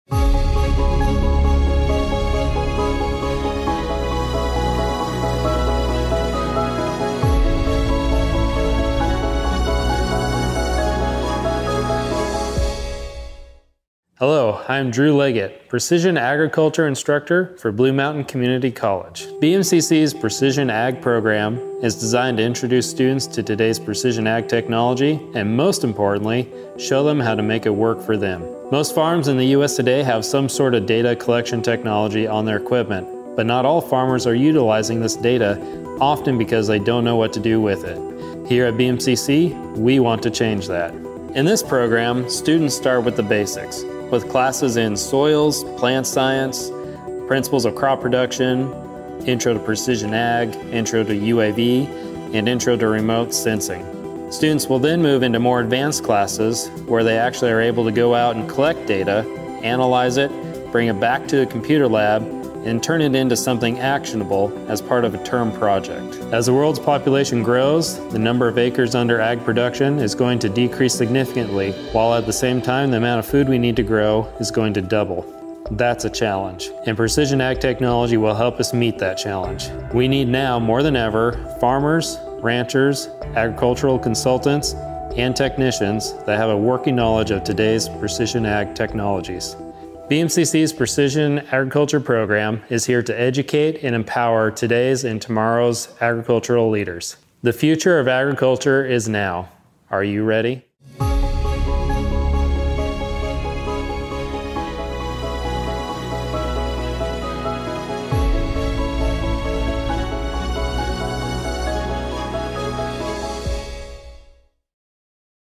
BMCC Precision Ag Series || BMCC Agriculture Program
Overview: A career in agriculture includes more options today than it ever has. Crops and livestock are not where it ends. Learn about GIS/GPS, electronics, genetics, animal nutrition, the environment, soils and chemistry, range management, modern business operations, ag issues and policy, economics, and so much more! BMCC's Ag Department can get you started on a degree or certificate, fill a gap in your knowledge, or help you learn skills that are necessary to be selected for jobs right here in our community. Learn more! Arrange a tour! Or, get started with your BMCC Ag classes, TODAY!